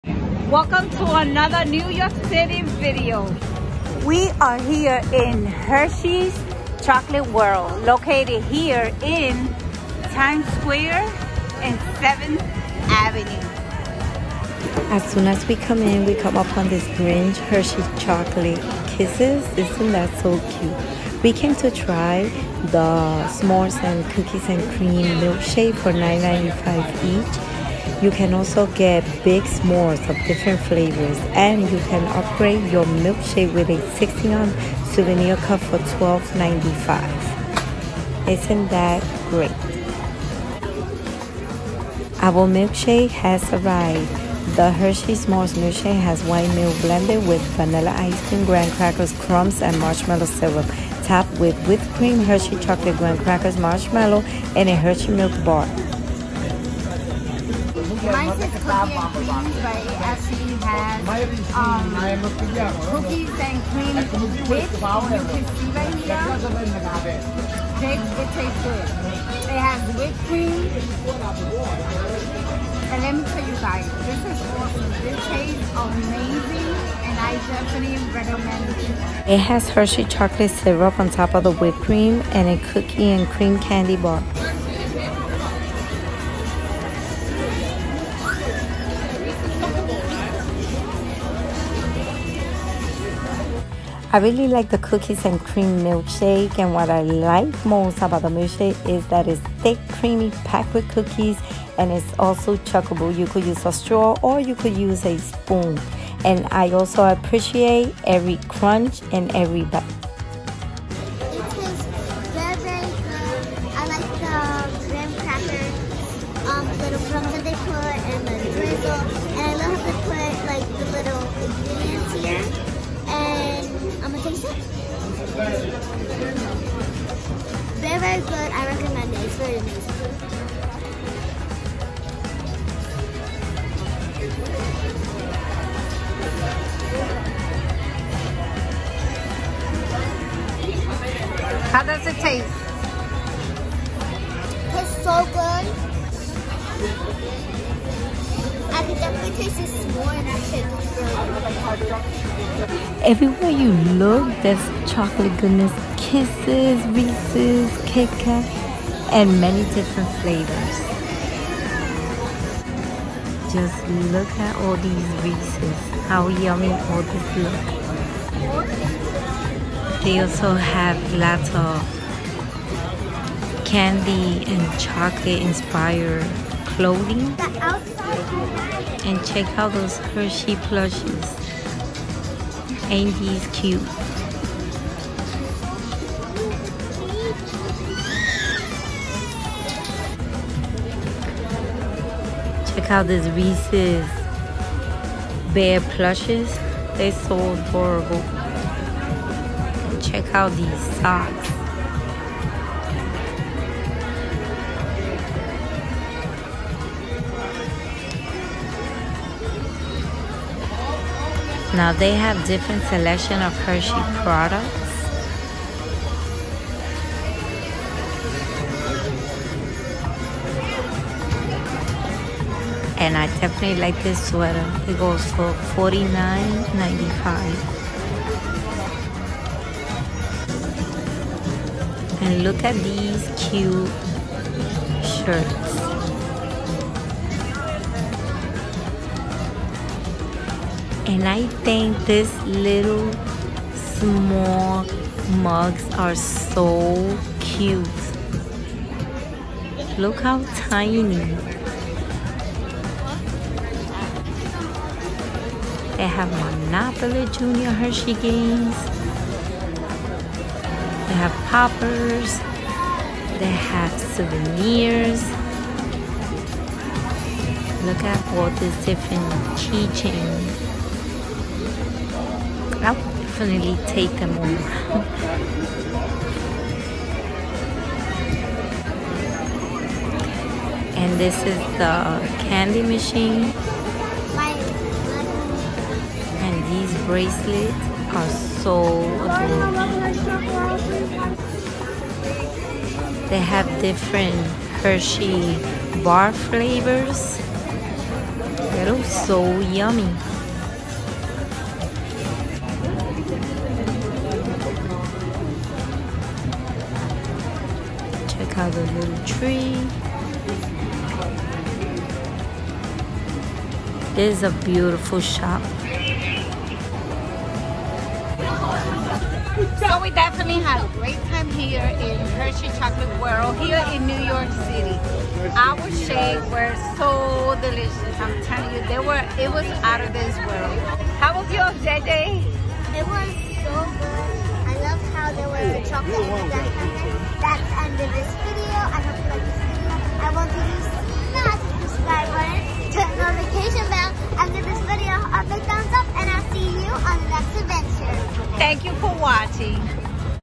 Hershey’s Chocolate World New York City Times Square. Hershey’s Shop Store Products Best Milkshakes
We visited Hershey’s Chocolate World in New York City Times Square. We try the s’mores milkshake and cookies and cream milkshake it was delicious They had many different chocolate flavors and candy selections. They had candy inspired clothing and many souvenirs. Hershey’s character plush toys are beautiful. Hershey’s Chocolate World merchandise are great value. We enjoy our visit to Hershey’s world chocolate